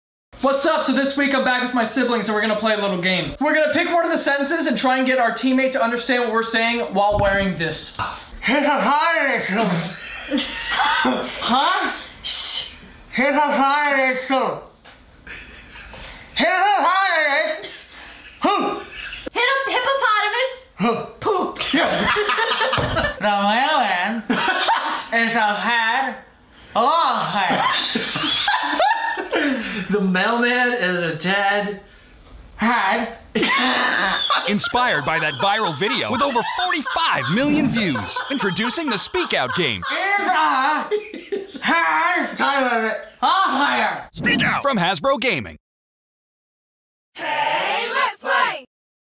Smyths Toys - Speak Out Game
Speak Out Game is laugh out loud hilarious. It's a ridiculously funny mouthpiece challenge game!

Friends and family will have great fun as they try to say different phrases while wearing a mouthpiece that won't let them shut their mouth. In the Speak Out game, players draw from the deck and read the phrase as best as they can, but it's not so easy to do when the mouthpiece is hindering them from forming words correctly. The timer counts down as players try to say phrases such as "he's my stealthy pet ferret name Garrett" OR "slow down, you careless clown". The laughter continues as players try to decipher what in the world their teammate is trying to say, and must correctly guess the phrase in order to earn the card. The team with the most cards at the end of the game wins.